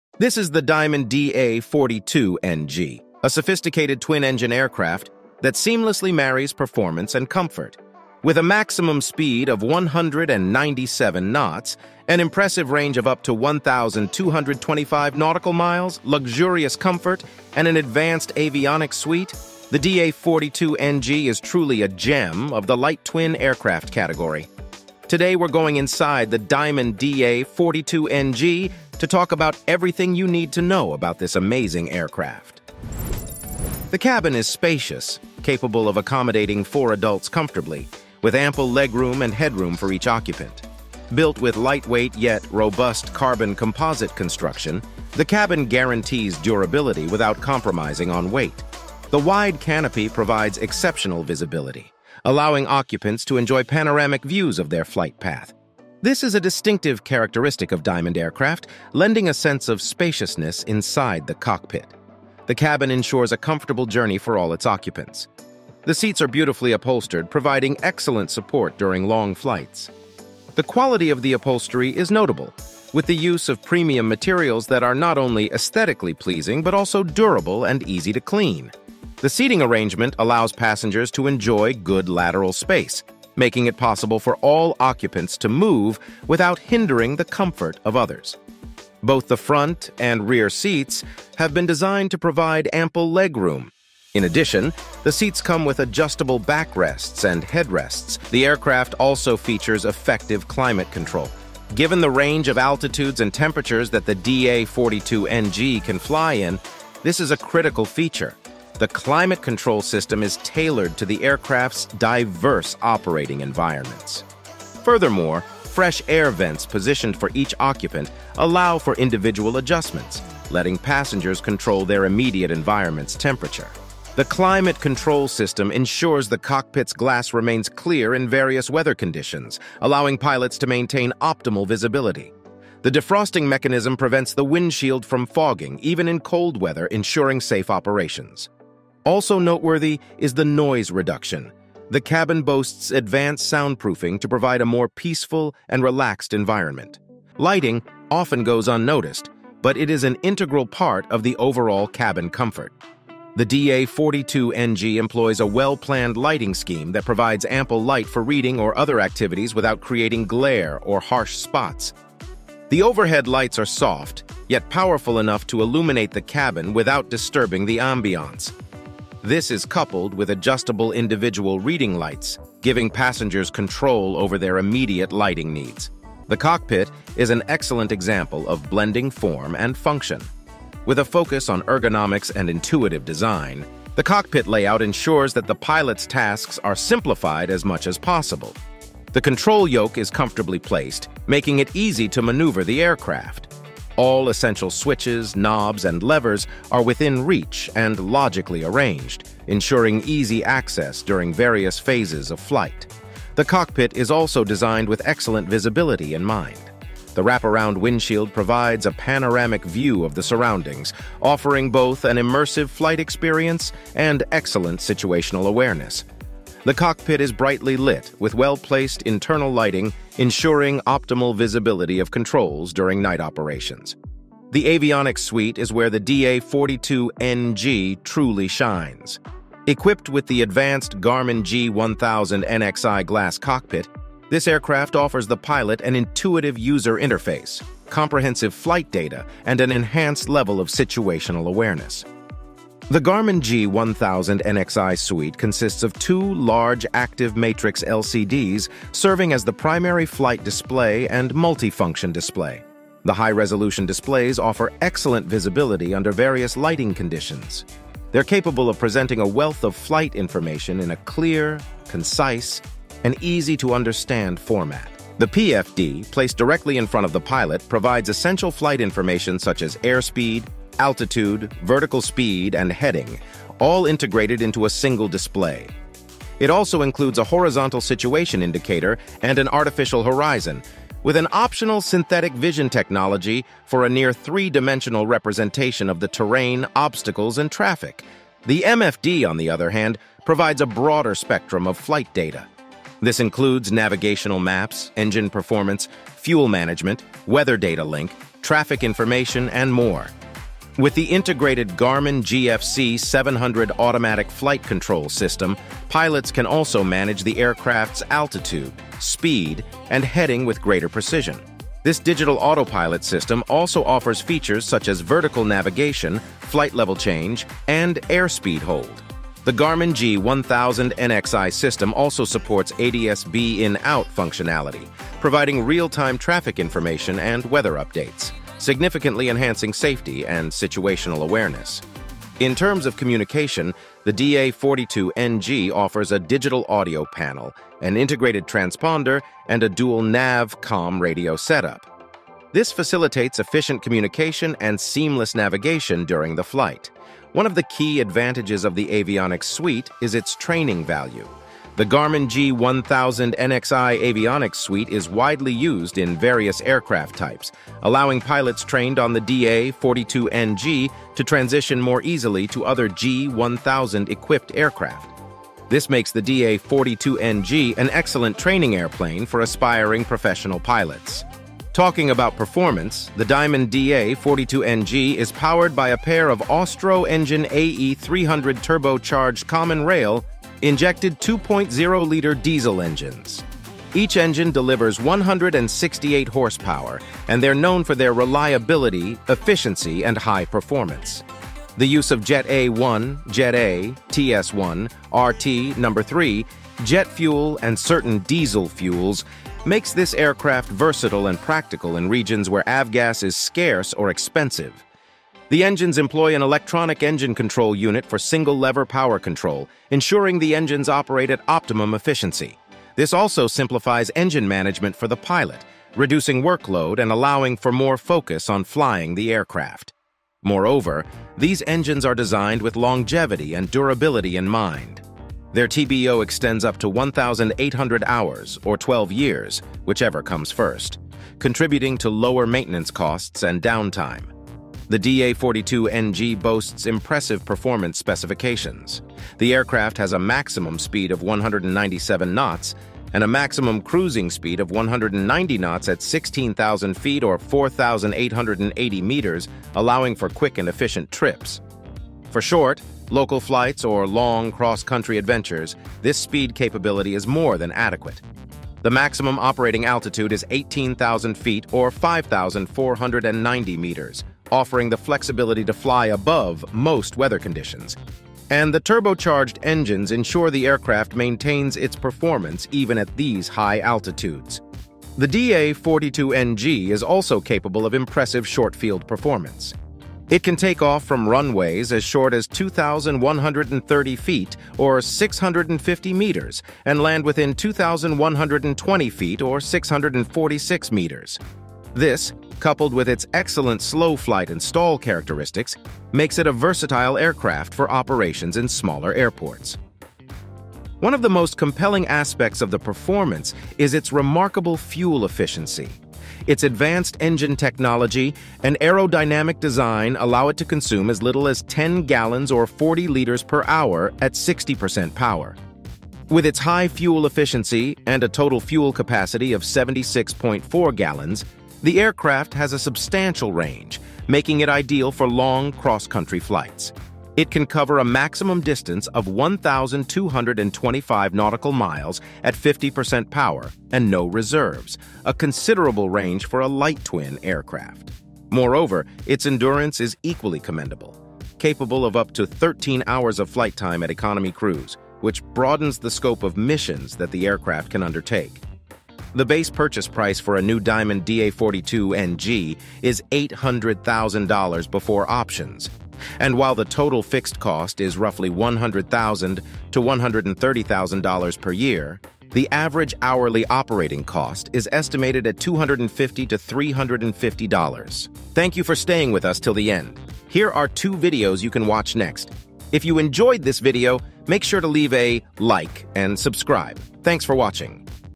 Inside The $1 Million Diamond DA42 NG
Inside The $1 Million Diamond DA42 NG Plane

This is the Diamond DA42 NG, a sophisticated twin-engine aircraft that seamlessly marries performance and comfort. With a maximum speed of 197 knots, an impressive range of up to 1,225 nautical miles, luxurious comfort, and an advanced avionics suite, the DA42 NG is truly a gem of the light-twin aircraft category.
The base purchase price for a new Diamond DA42 NG is $800,000 before options, and while the total fixed cost is roughly $100,000 to $130,000 per year, the average hourly operating cost is estimated at $250 to $350.
Here is everything you need to know about the Diamond DA42 NG.

TIMESTAMPS
0:00 - Intro
0:33 - Cabin
2:55 - Cockpit
6:11 - Performance
9:09 - Prices
9:29 - Outro